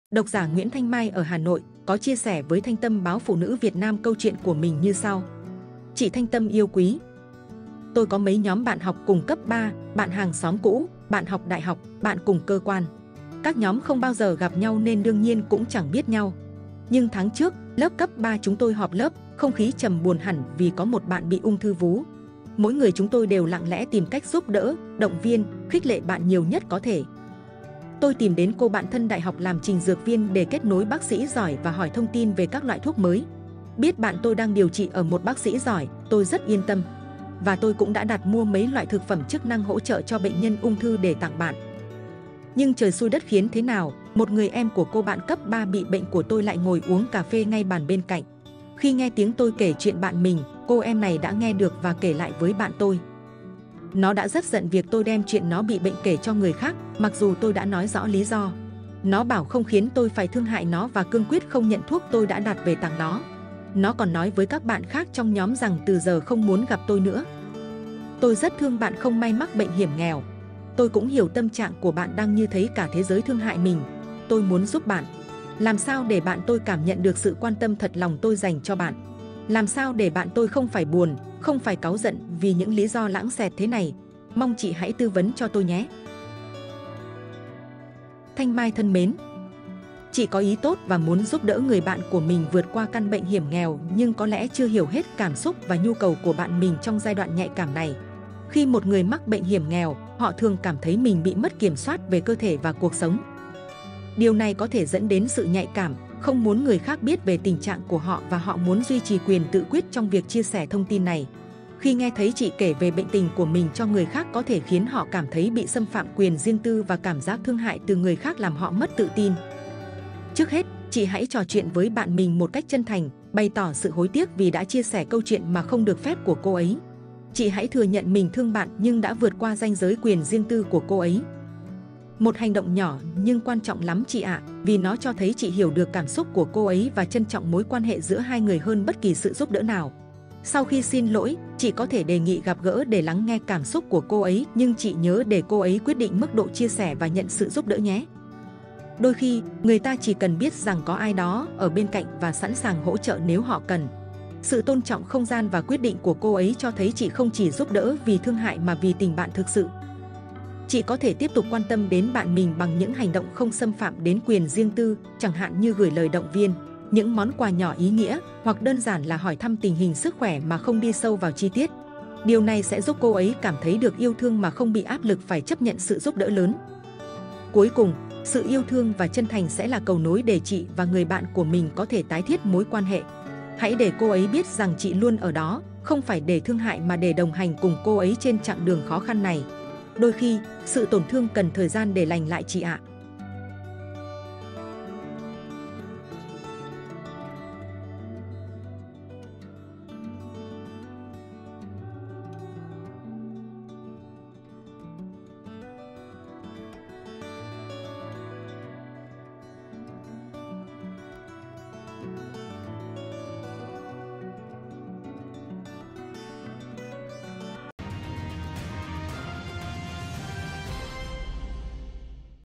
Bị giận vì đem chuyện bạn mắc bệnh nan y kể cho người khác
Chị Thanh Tâm yêu quý!
Tôi có mấy nhóm bạn học cùng cấp 3, bạn hàng xóm cũ, bạn học đại học, bạn cùng cơ quan… Các nhóm không bao giờ gặp nhau nên đương nhiên cũng chẳng biết nhau. Nhưng tháng trước, lớp cấp 3 chúng tôi họp lớp, không khí trầm buồn hẳn vì có một bạn bị ung thư vú. Mỗi người chúng tôi đều lặng lẽ tìm cách giúp đỡ, động viên, khích lệ bạn nhiều nhất có thể.

Tôi tìm đến cô bạn thân đại học làm trình dược viên để kết nối bác sĩ giỏi và hỏi thông tin về các loại thuốc mới. Biết bạn tôi đang điều trị ở một bác sĩ giỏi, tôi rất yên tâm. Và tôi cũng đã đặt mua mấy loại thực phẩm chức năng hỗ trợ cho bệnh nhân ung thư để tặng bạn.

Nhưng trời xui đất khiến thế nào, một người em của cô bạn cấp 3 bị bệnh của tôi lại ngồi uống cà phê ngay bàn bên cạnh. Khi nghe tiếng tôi kể chuyện bạn mình, cô em này đã nghe được và kể lại với bạn tôi.

Nó đã rất giận việc tôi đem chuyện nó bị bệnh kể cho người khác, mặc dù tôi đã nói rõ lý do. Nó bảo không khiến tôi phải thương hại nó và cương quyết không nhận thuốc tôi đã đặt về tặng nó. Nó còn nói với các bạn khác trong nhóm rằng từ giờ không muốn gặp tôi nữa.

Tôi rất thương bạn không may mắc bệnh hiểm nghèo. Tôi cũng hiểu tâm trạng của bạn đang như thấy cả thế giới thương hại mình. Tôi muốn giúp bạn. Làm sao để bạn tôi cảm nhận được sự quan tâm thật lòng tôi dành cho bạn? Làm sao để bạn tôi không phải buồn, không phải cáu giận vì những lý do lãng xẹt thế này? Mong chị hãy tư vấn cho tôi nhé!

Nguyễn Thanh Mai (Hà Nội)